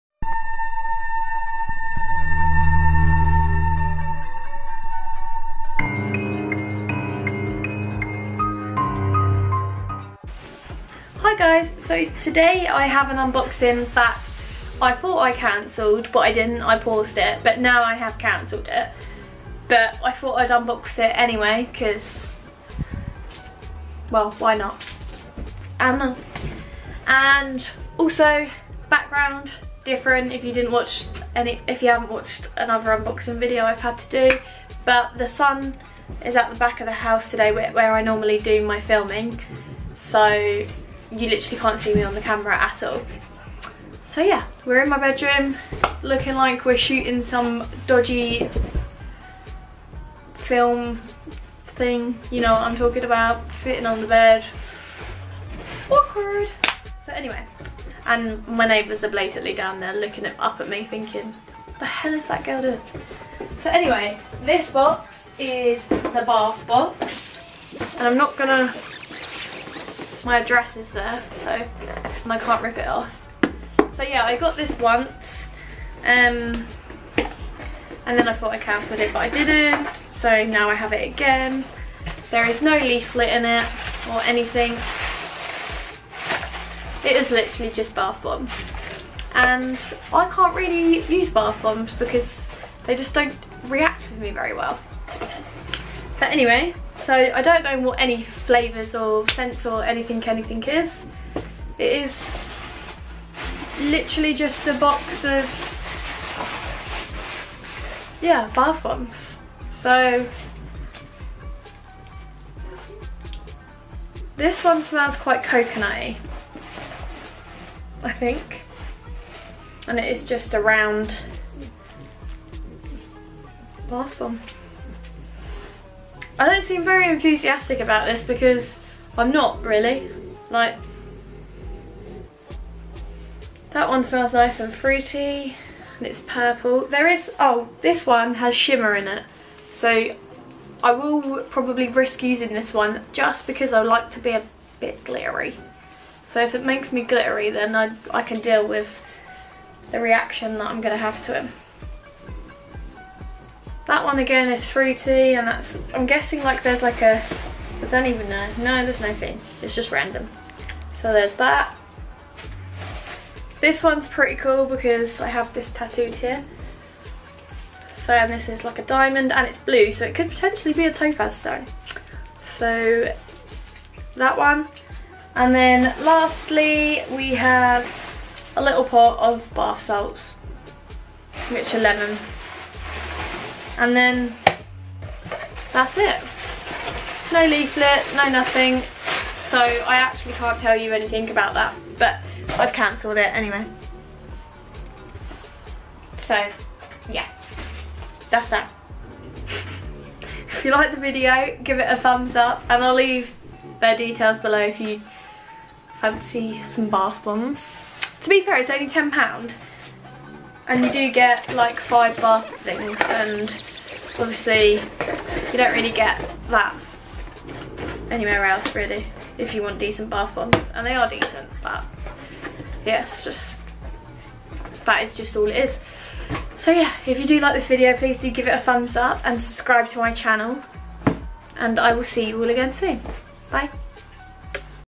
BATH BOX SUBSCRIPTION BOX UNBOXING JUNE 2019 ❤
literally what it says on the box .. a box for your bath needs